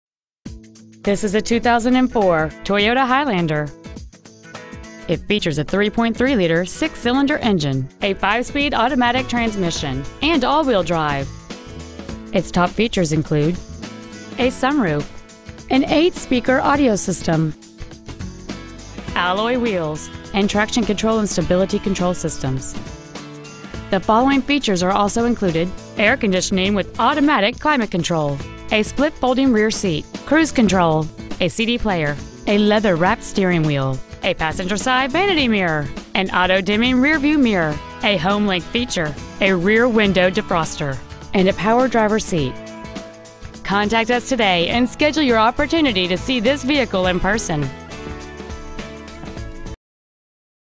Pre-Owned 2004 Toyota Highlander Napervillle IL
We have been honored to serve the Napervillle IL area , we promise that your experience at our dealership will exceed your expectations ! Year : 2004 Make : Toyota Model : Highlander Engine : 3.3l v6 smpi dohc Trans . Napervillle , IL 60540